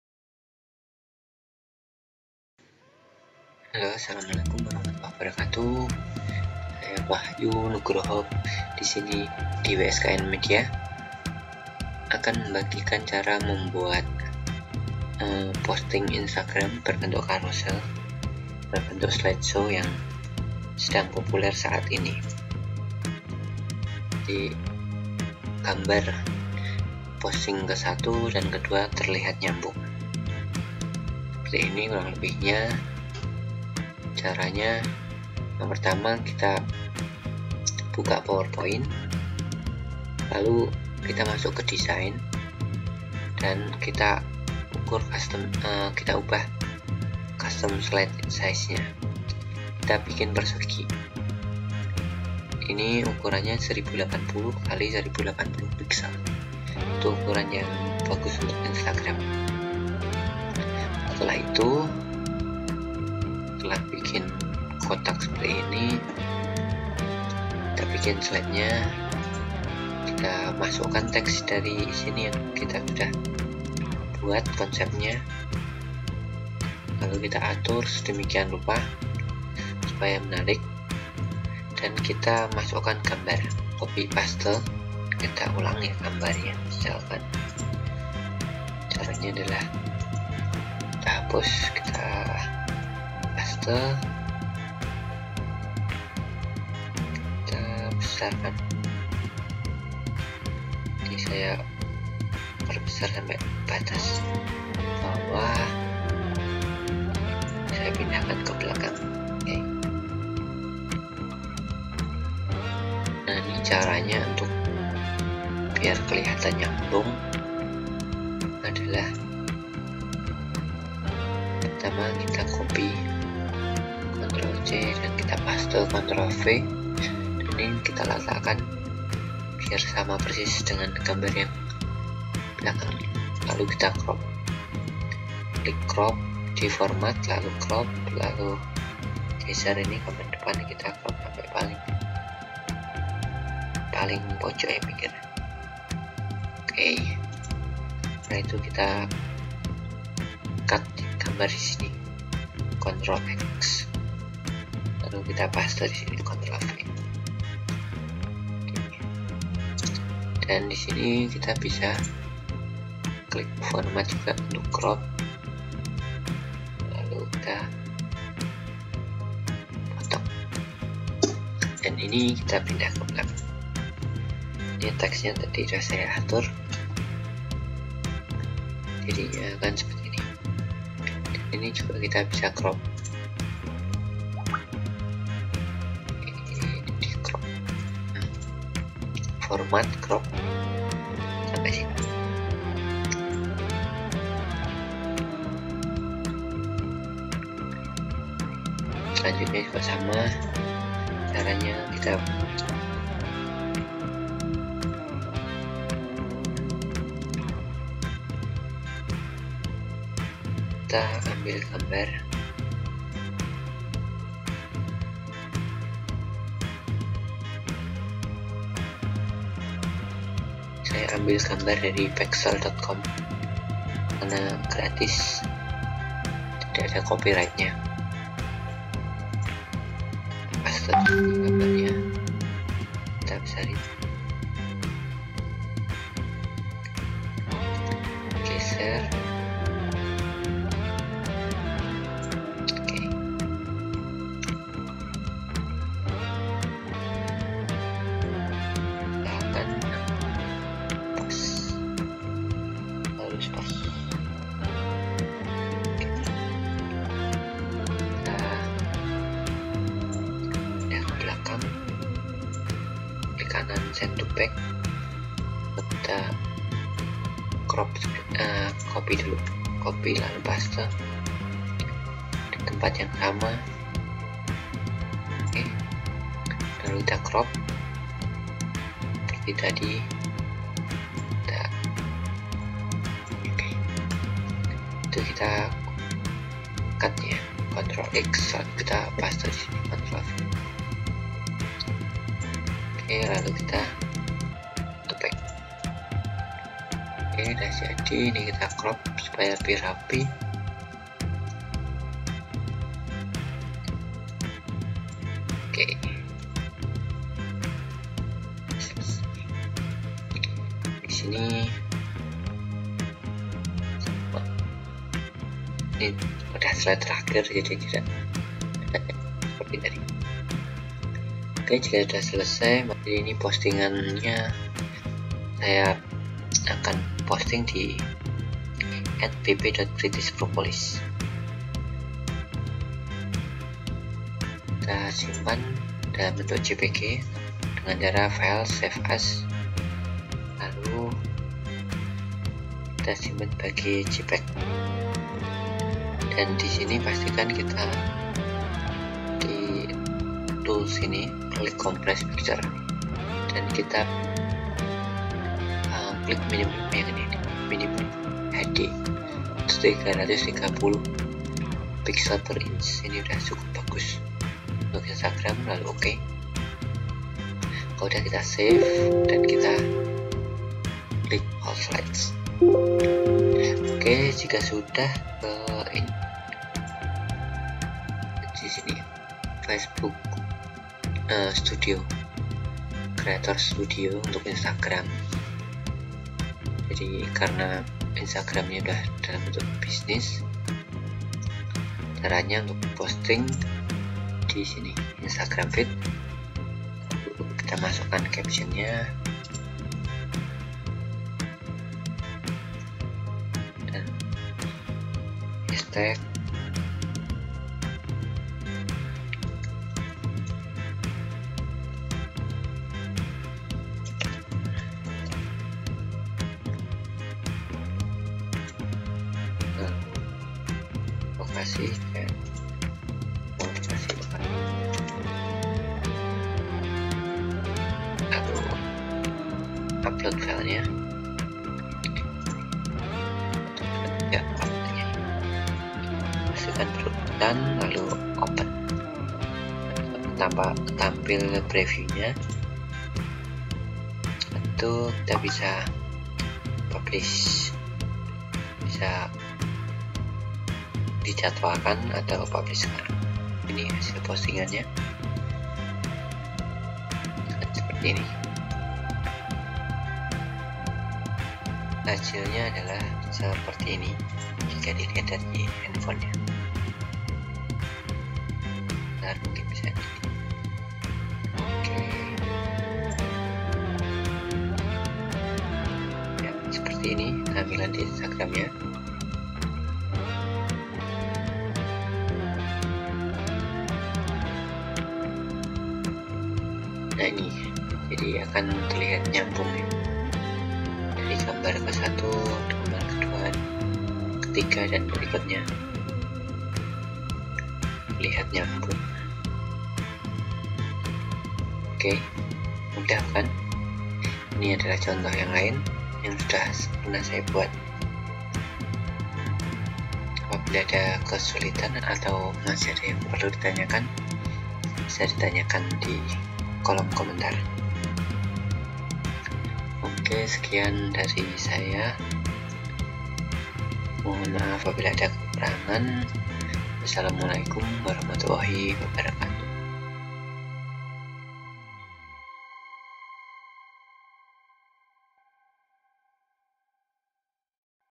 [WSKN Media] #1 TUTORIAL POWER POINT - CARA MUDAH MEMBUAT SLIDESHOW NYAMBUNG Carousel di Instagram
Di video ini akan diberikan cara / tutorial mudah cara membuat Carousel album nyambung Instagram pakai power point yang sedang populer saat ini..
Tujuannya adalah supaya foto ke satu dan selanjutnya bersambung,

Banyak cara yang dapat dilakukan akan tetapi, pada video ini dijelaskan pembuatan menggunakan power point, supaya lebih mudah di lakukan untuk semua orang.

Semoga bermanfaat!
Share ke teman bila bermanfaat,
Silakan komen untuk kritik dan saran, atau request tutorial lain, kalau bisa akan coba saya buat kan :D

Terima kasih